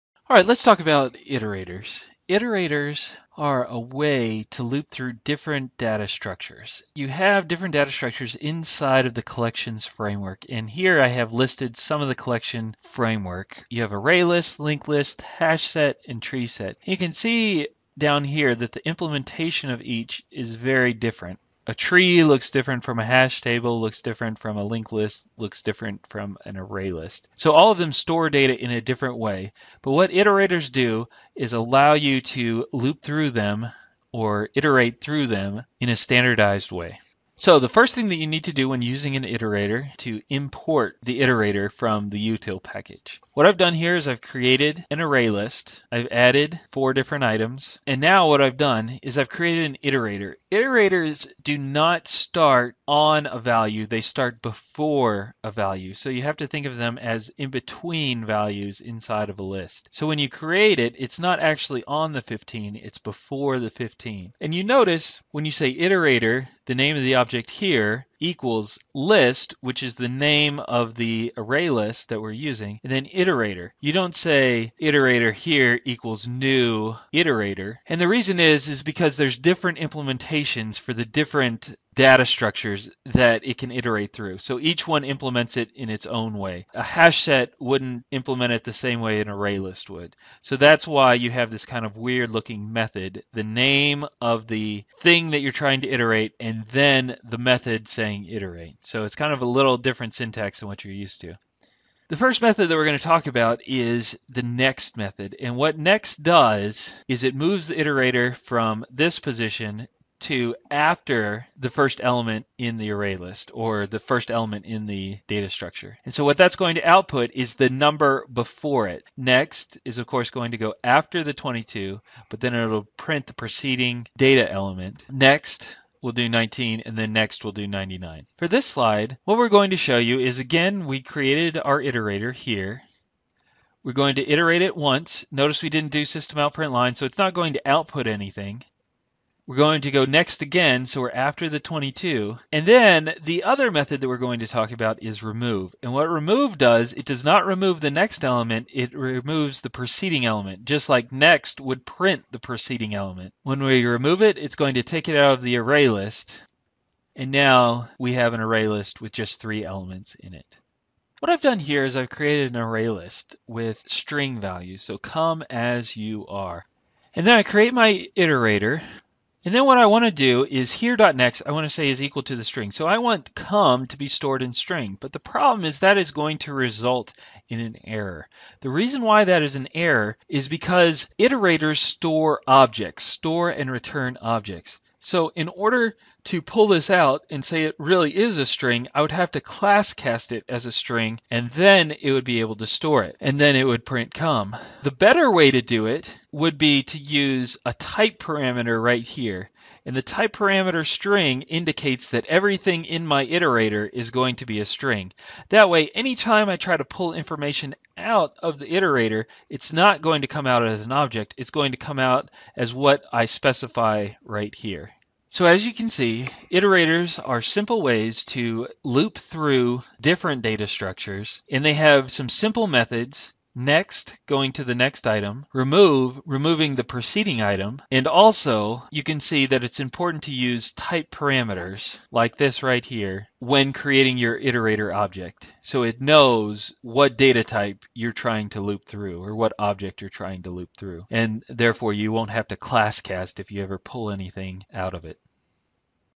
Iterators Part 1 (Java)
This video looks at how iterators are created and why they are used.